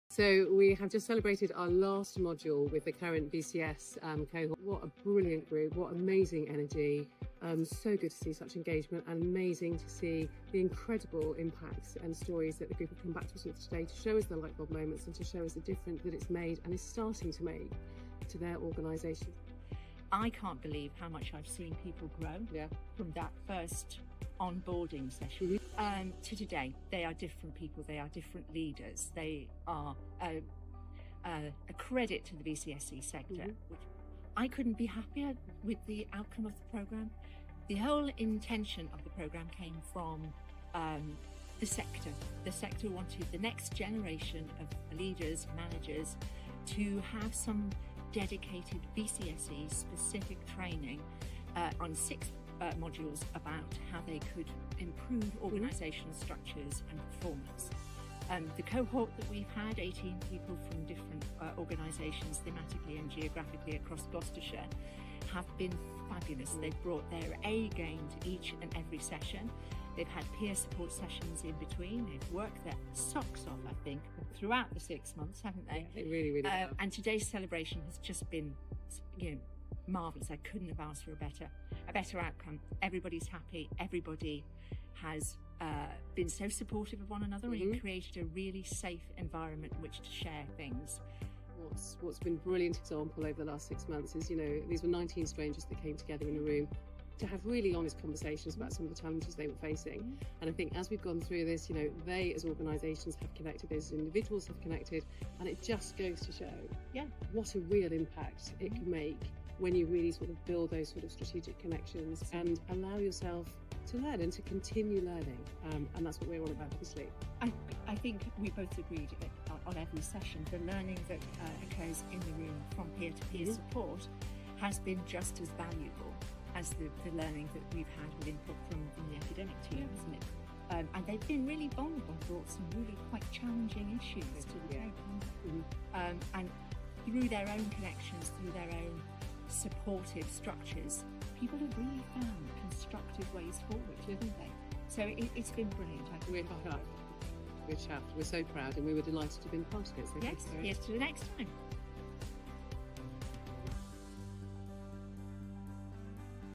University of Gloucestershire | Gloucestershire VCS Alliance | LeaderLab | 2024 Cohort
Gloucestershire VCS Alliance and University of Gloucestershire delivered a six-month leadership, enterprise and professional development programme for VCSE sector groups & organisations in Gloucestershire.

The programme compromised six full-days of teaching at the Gloucestershire Business School, made up of directed learning and group work specifically designed for VCSE sector groups/organisations. Attendees also benefited from a bespoke peer-to-peer community.

Hear Alex Cottrell, Head of External Relationships at University of Gloucestershire, and Cate Hemmingway, Head of Sector Innovation at Gloucestershire VCS Alliance, reflect on how the programme delivered against the objectives.

If you want to find out more about Gloucestershire VCS Alliance visit their

If you want to discuss how LeaderLab can help your organisation, or explore other bespoke leadership training on offer from University of Gloucestershire, email the team on .